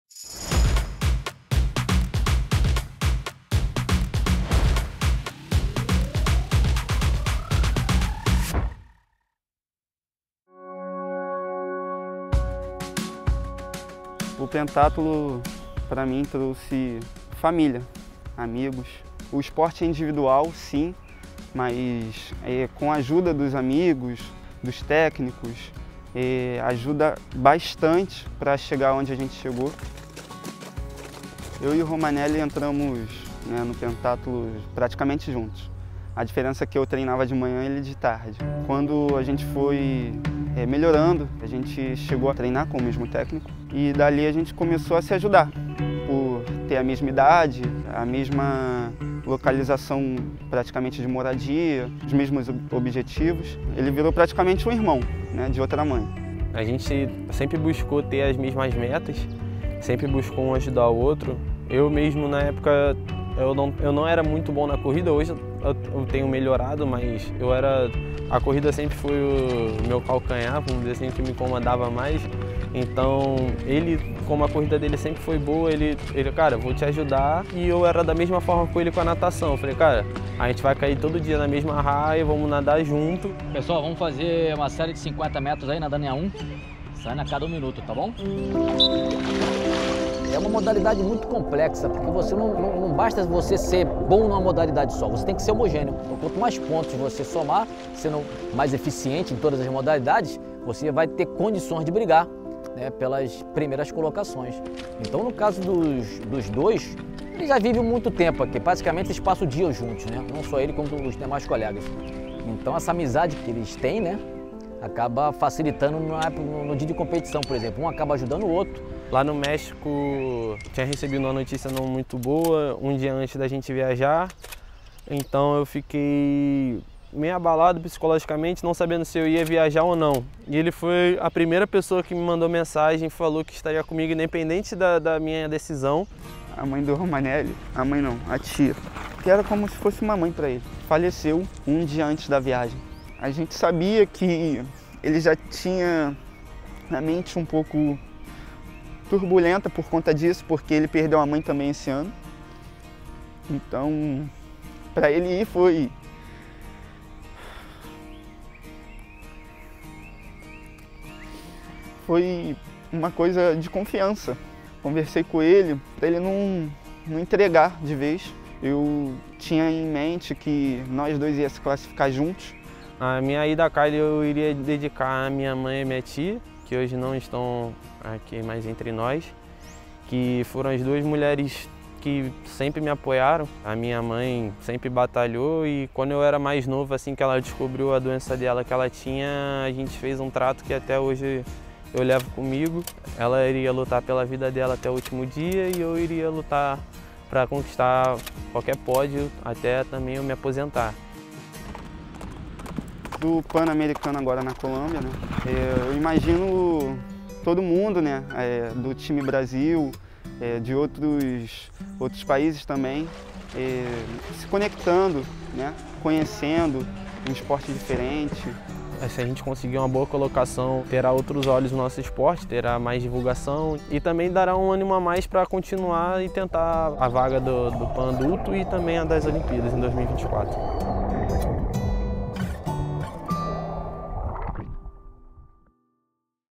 Conheça a história de Matheus Nobre e Matheus Romanelli, jovens promessas do pentatlo moderno
Matheus Nobre e Matheus Romanelli começaram no pentatlo moderno juntos e agora vão representar o Time Brasil em Cali (Colômbia). Conheça um pouco da história desses atletas que estão na primeira edição dos Jogos Pan-americanos Júnior Cali 2021, que acontece de 25 de novembro a 5 de dezembro, e reunirá mais de 3.800 atletas, de 41 países (incluindo o Brasil).

O Time Brasil estará representado por 358 atletas, de 38 modalidades, e vai em busca de vagas para os Jogos Pan-americanos Santiago 2023.